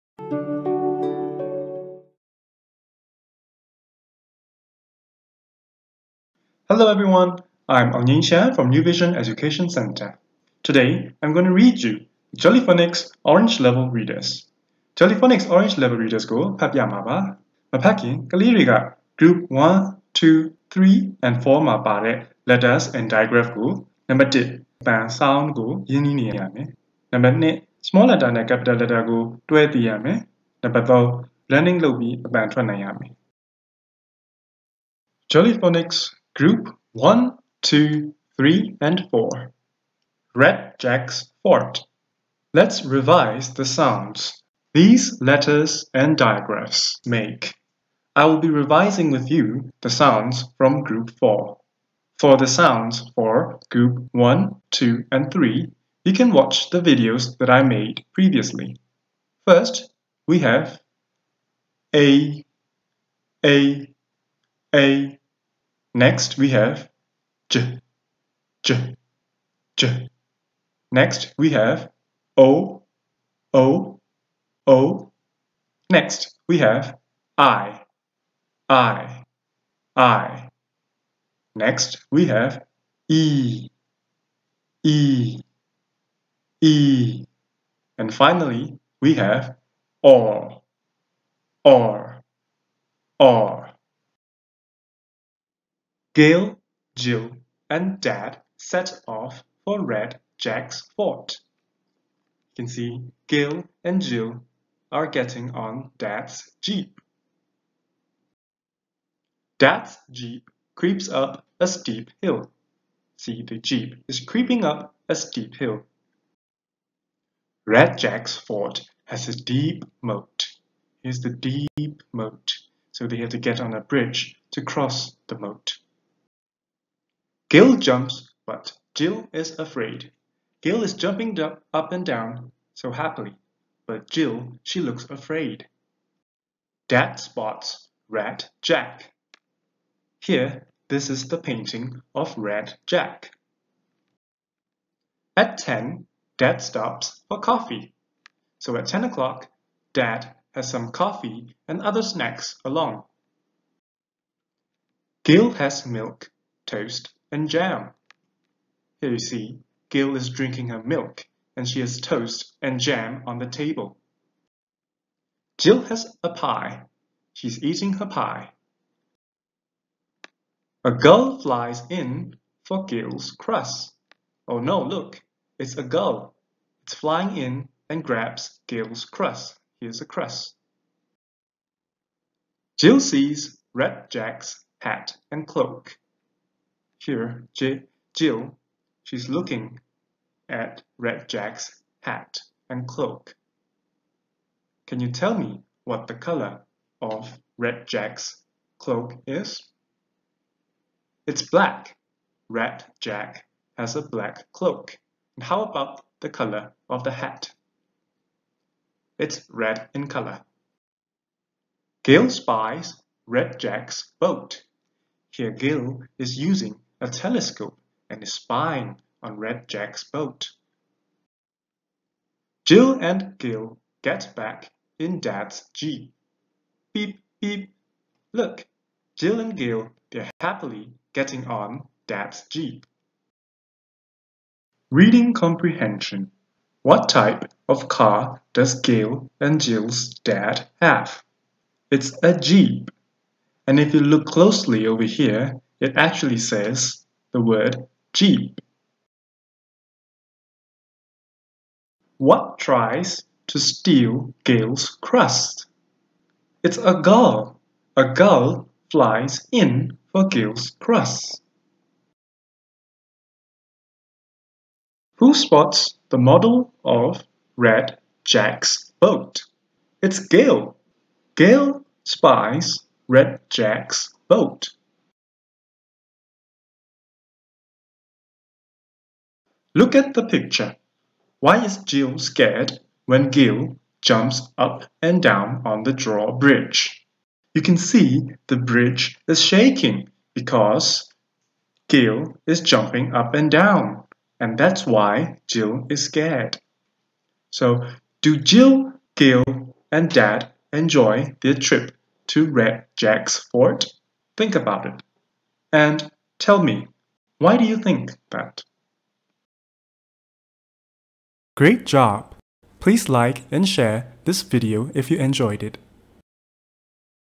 Red Jack's Fort JollyPhonicsReaders OrangeLevel Group1,2,3,4
Red Jack's Fort ကို ဖတ်ပြထားပါတယ်
Before tackling these Orange Level  Readers, a child will need to be able to do the following:
ဤစာအုပ်ကို မဖတ်မီ ကလေးသည် -

Say the sounds made by the lower-case letters, digraphs and capital letters;
Group1,2,3,4 မှ  Letters and Digraphs တို့၏ အသံကို သိရမည်

Match the lower-case letters to the corresponding capital letters
Group1,2,3,4 မှ  Letters တို့၏ သက်ဆိုင်ရာ capital letters တို့ကို မှန်ကန်စွာ ယှဉ်တွဲနိုင်ရမည်

Read (blend) regular words containing these letter sounds.
Group1,2,3,4 မှ  Letters and Digraphs တို့၏  အသံတို့နှင့် စာလုံးပေါင်းဖတ်နိုင်ရမည်